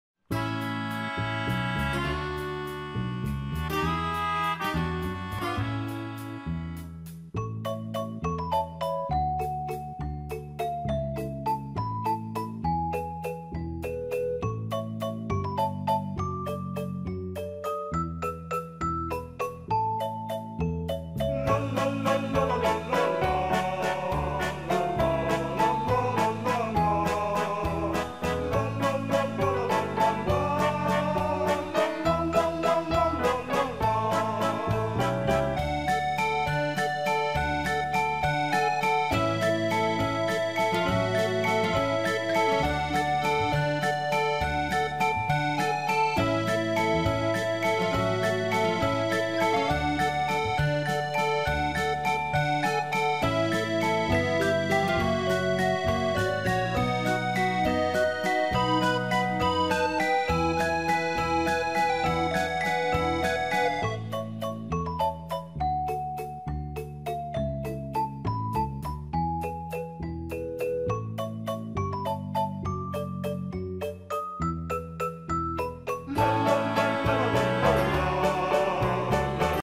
Unboxing TOYS Review/Demos - lego at the fair merry go round animal elephant
Grandma toystore brings to you the Unboxing TOYS Review/Demos -  lego at the fair merry go round animal elephant